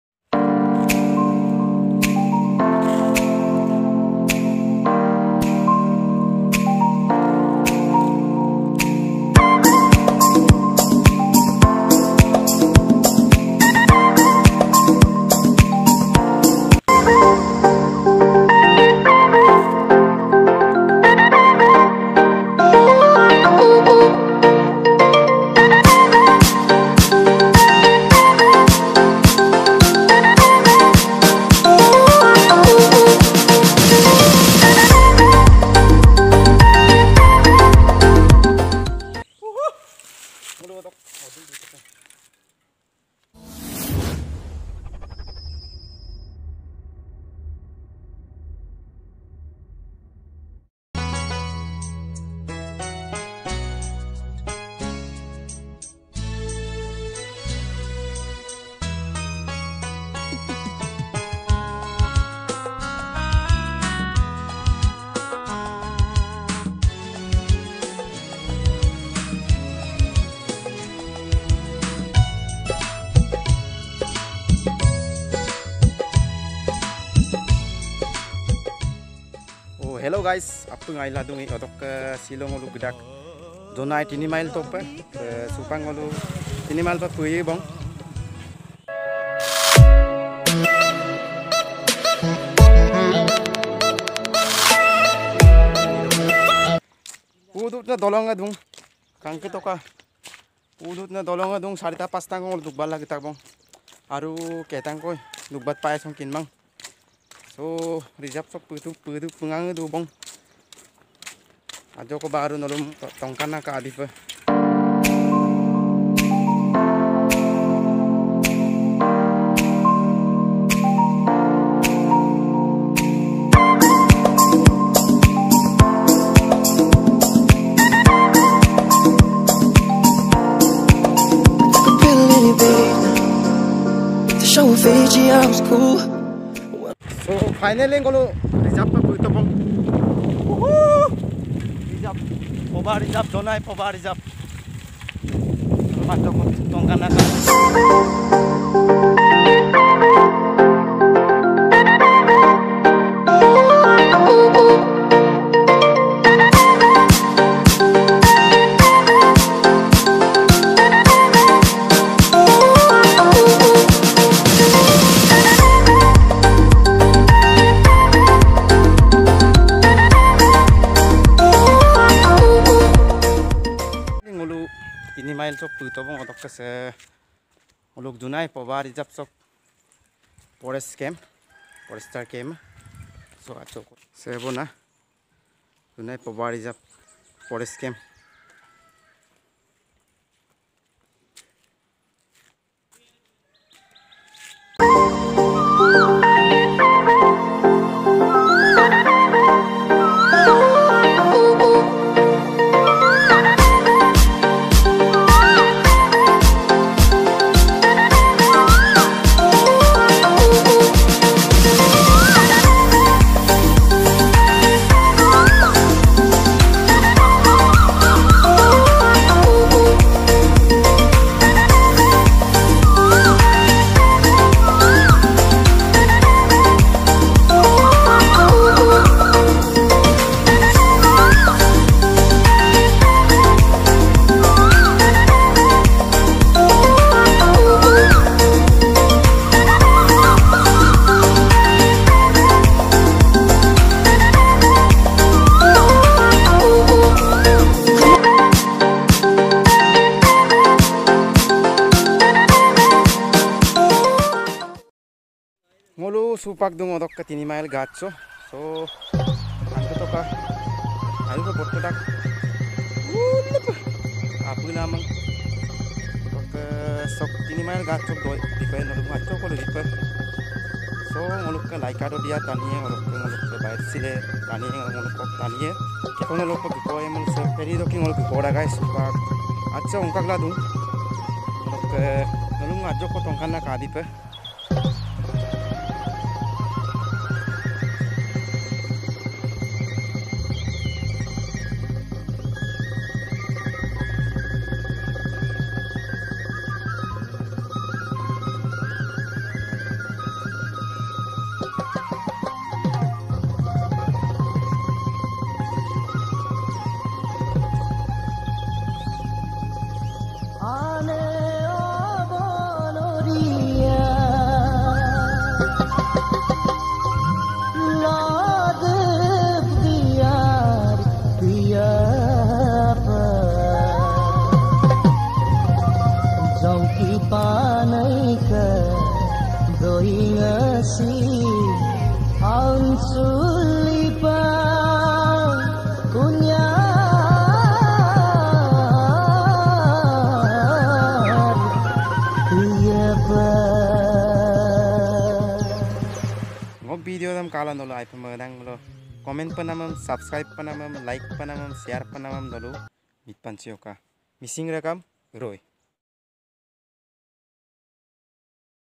Jonai Poba Reserve Forest || Kokoroli Vloge
[ JONAI POBA RESERVE ]

Jonai poba reserve forest
poba reserve forest is situated in jonai
sub-division of Dhemaji district of Assam.
Poba reserve forest covers an Area of 10,221 Hactares.
Poba is one of the Richest Rain forest of
Interns of flora and founa found in it and
can be very well termed as a Biodiversity
hotspot.
Highest Temperature record is 36 Degree
C in Summer and 7 degree in winter
Towards horth are the foothills of Himalayan Range in Arunachal Pradesh.
Towards East and South are the siyang,
Dibang and Lohit river.
Poba rain forest is an important Elephant
Corridor linking the Foothills of Arunachal
and dibru saikhowa national park.